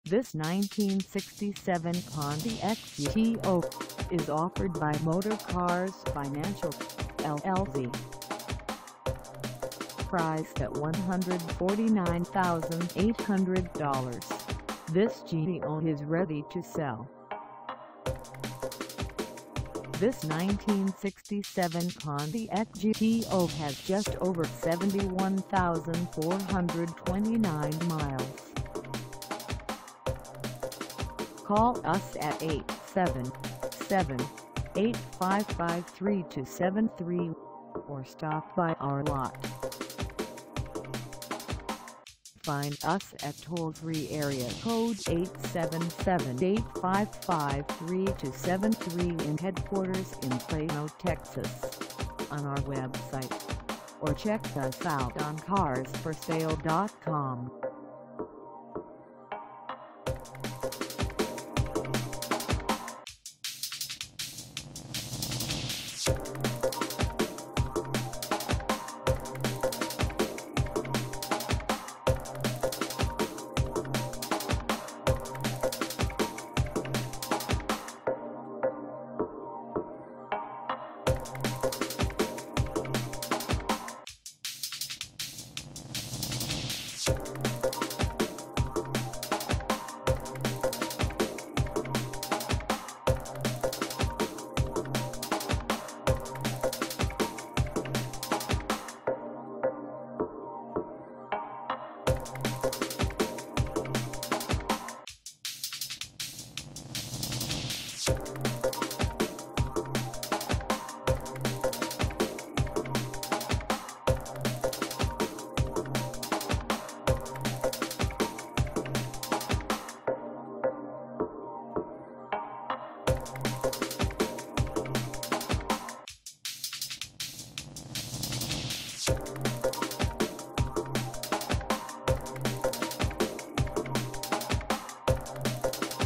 1967 Pontiac GTO Concours Gold 1st Place Winner Convertible
This 1967 Pontiac GTO Concours Gold 1st Place Winner Convertible is for sale in Headquarters in Plano, TX 75086 at Motorcars Financial, LLC.
Engine: Unspecified 400 H/O 360HP
Transmission: Unspecified
Color: White
Mileage: 71429
Contact Motorcars Financial, LLC Toll Free Headquarters in Plano, TX 75086 to test drive this 1967 Pontiac GTO Concours Gold 1st Place Winner Convertible today.